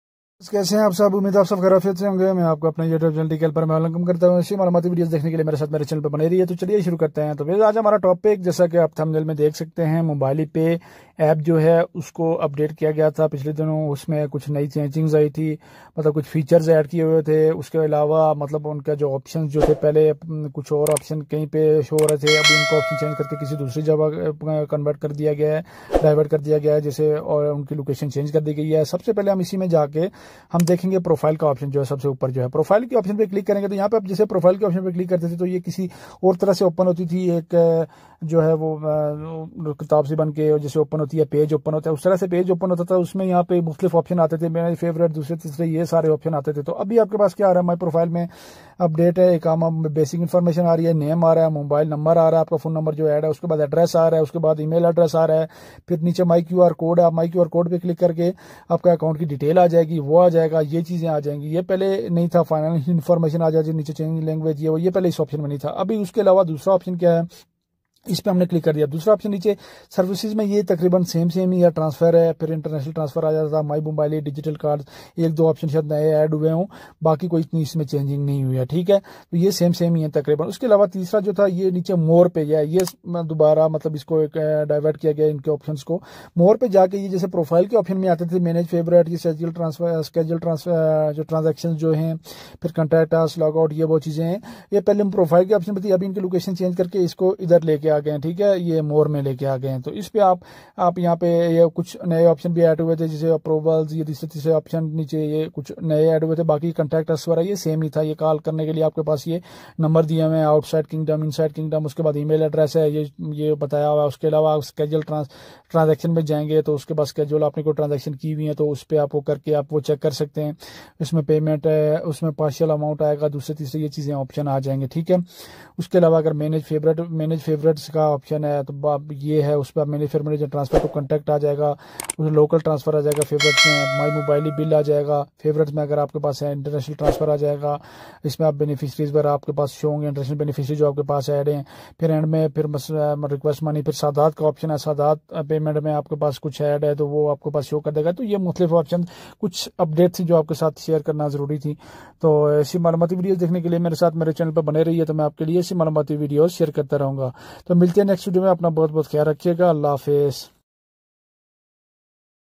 Mobily Pay App New Updates | Mobily Pay App Ki Taraf Se New Updates Agai Hain | TK HELPER
Mobily Pay App New Updates, is video main hum daikhn gay k mobily pay ki taraf se kia nahi updates agai hain.